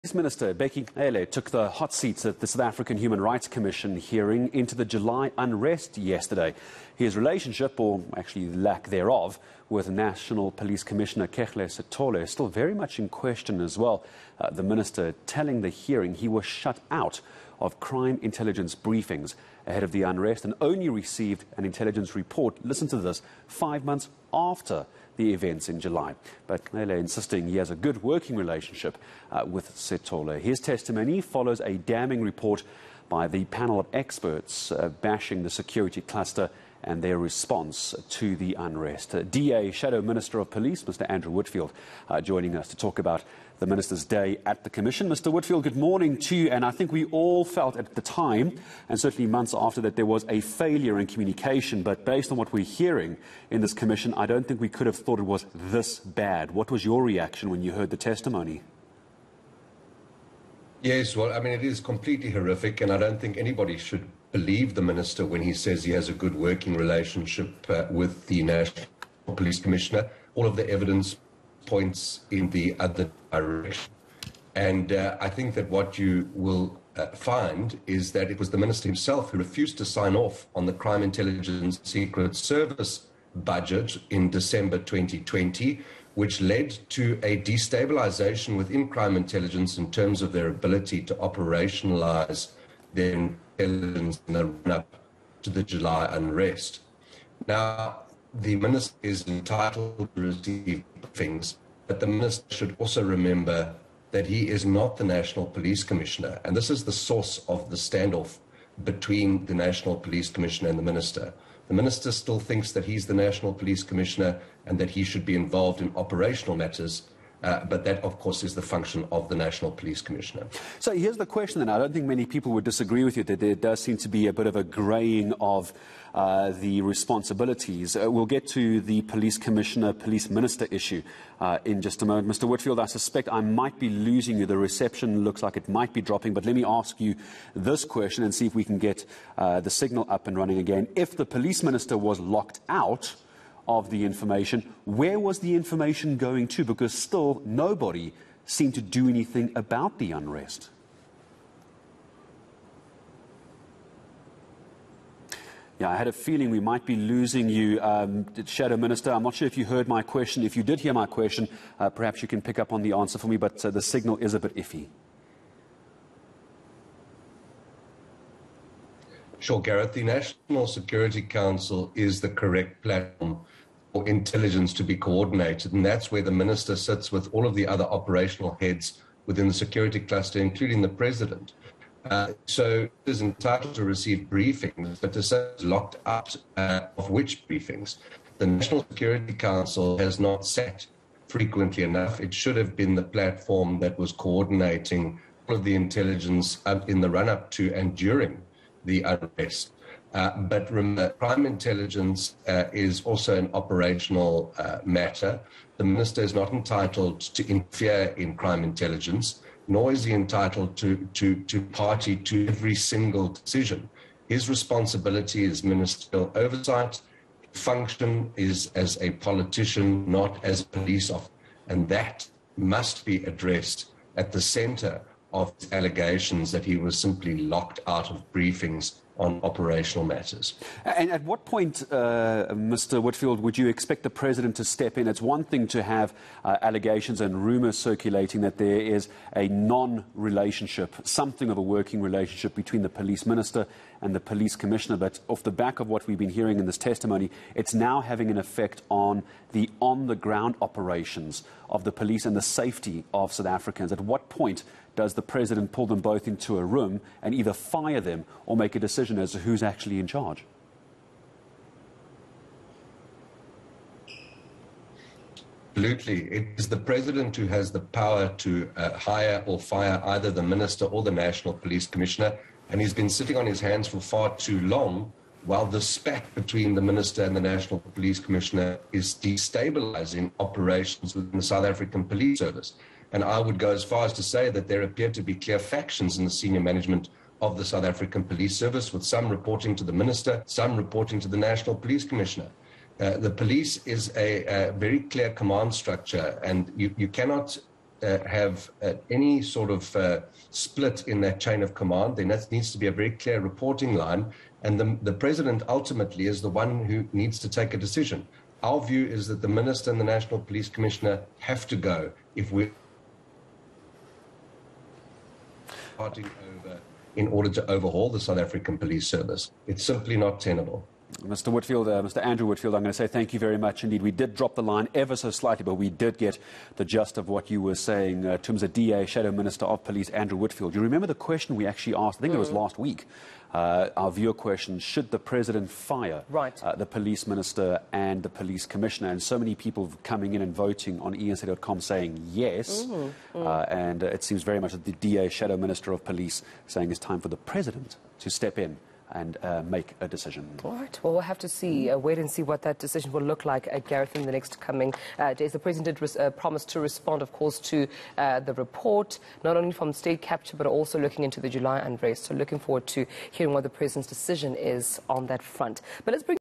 July Unrest | Discussion | Cele shut out of intelligence briefings
Police Minister Bheki Cele took the hot seat at the South African Human Rights Commission hearing into the July unrest. DA Shadow Minister of Police, Andrew Whitfield speaks to eNCA Courtesy